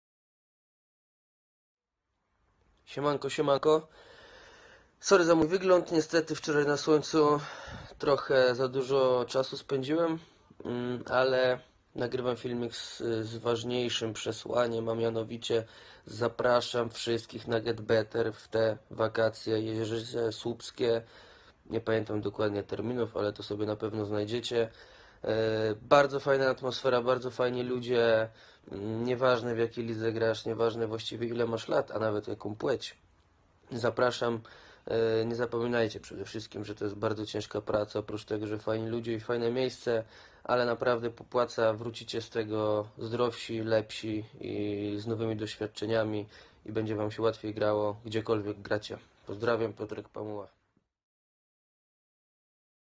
Zaproszenie GETBETTER - Piotr Pamuła
Czas między zakończeniem, a początkiem nowego sezonu to nawet 4 miesiące.

Naszym celem jest stworzenie miejsca w którym zawodnicy mają możliwość rozwoju swoich umiejętności w jaki najefektywniejszy sposób. W jednym miejscu będziesz mógł ciężko pracować na sukces w nowym sezonie. Wierzymy, że ciężką pracą jesteś w stanie zajść daleko, a jak daleko to zależy tylko od Ciebie!